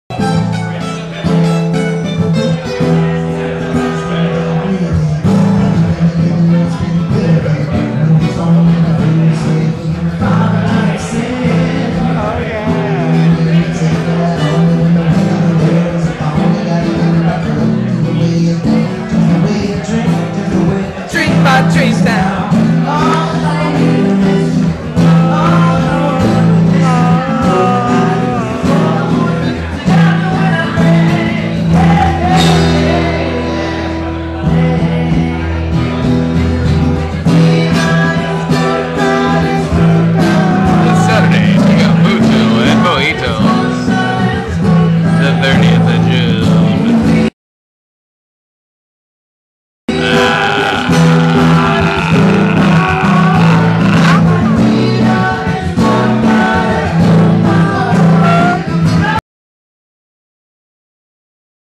Bucho @ Mojitos Saturday June 30th
Come check out the full band this Saturday June 30th @ Mojitos in San Francisco. We'll be playin 2 sets, lil DJ love in between from the whooligan or venetti, all family love. Get there early, Saturday's are killin at Mojitos yall! Mucho Bucho Amore